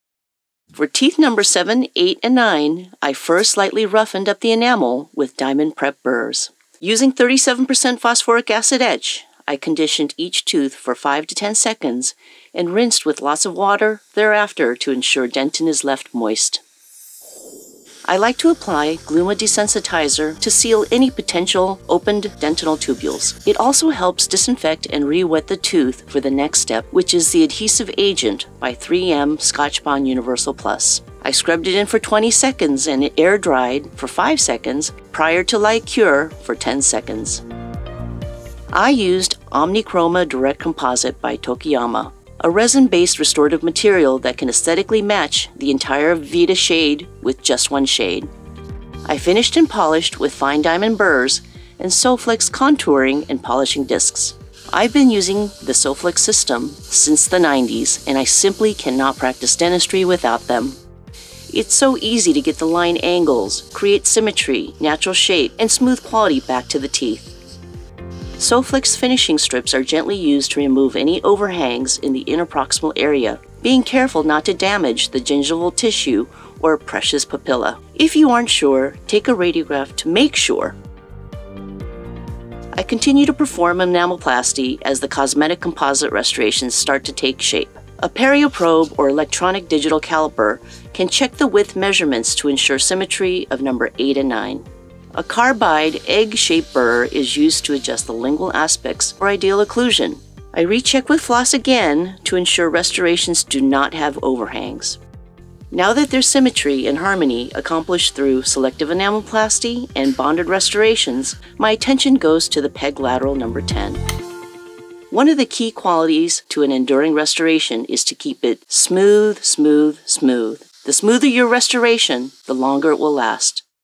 Selective COSMETIC Enameloplasty in the ANTERIOR Teeth
For this Part 2 of 4 clinical episodes, I used Omnichroma® bonding composite material (Tokuyama Corp.; Shunan, Japan) on teeth #7–9 to restore symmetry to the facial surfaces. Then, I performed enameloplasty on teeth #6–9 and 11 to improve the contour of the incisal edges. On the lower arch, I performed enameloplasty on teeth #22–27, contouring the teeth to a more even contour.

⏱ TIMESTAMPS⏱
0:00 - Intro
0:17 - Gluma desensitizer
0:36 - Enameloplasty
1:22 - Conclusion
1:53 - Bonus material